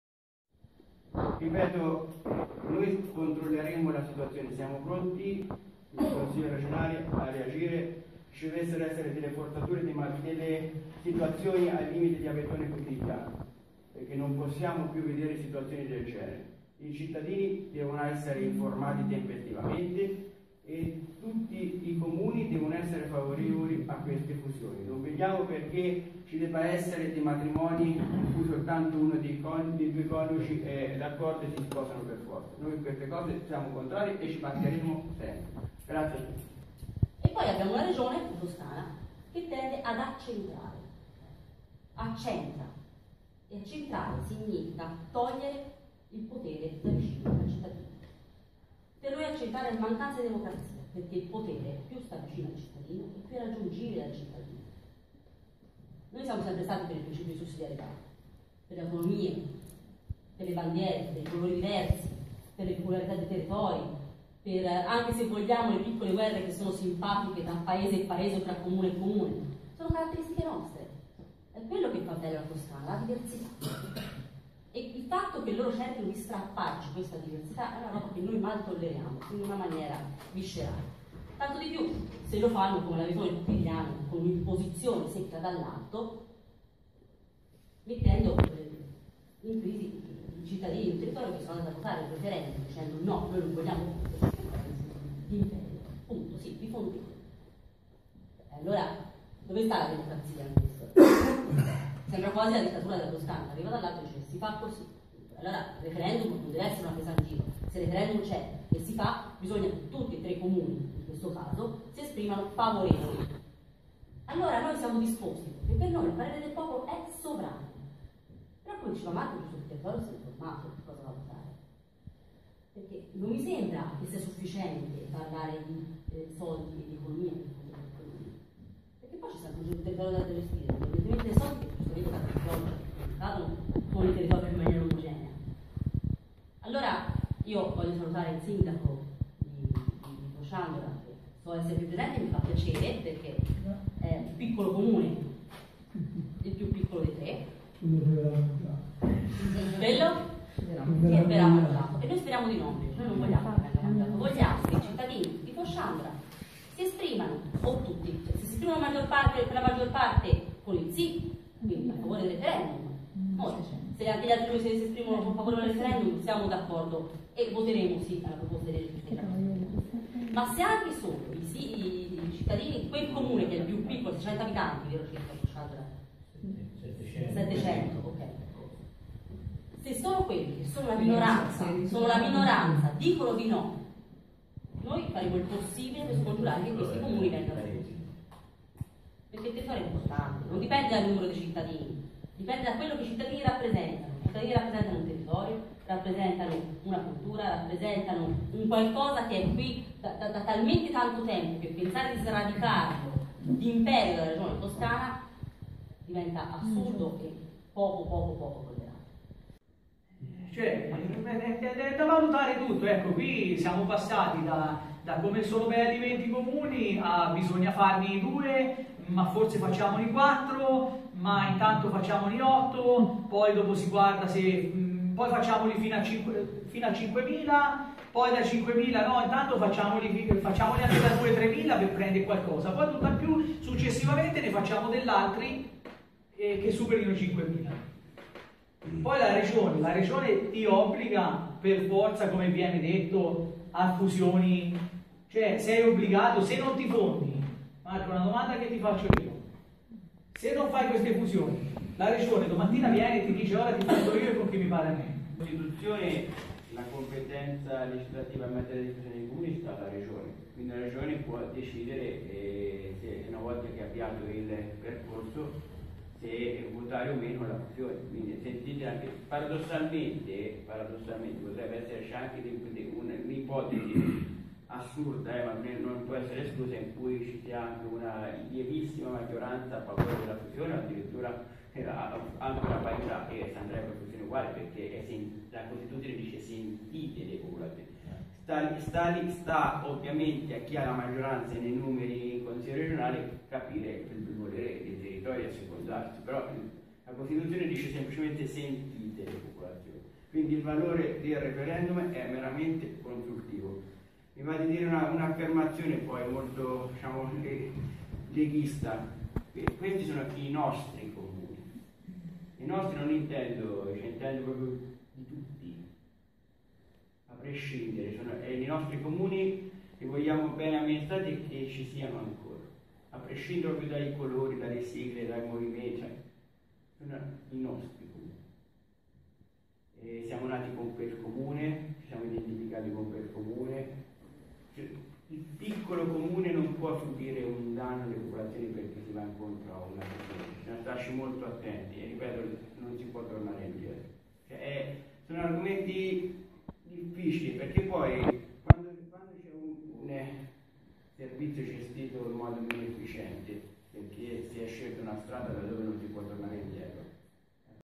Fusione dei Comuni Pieve Fosciana-Fosciandora-San Romano
Marco Casucci, Elisa Montemagni e Simone Simonini (Lega Nord): “La fusione dei comuni di Pieve Fosciana, Fosciandora e San Romano in Garfagnana nasconde interessi economici. Diciamo NO alla fusionomania del pd e Sì alle decisioni assunte dalla maggioranza dei cittadini.”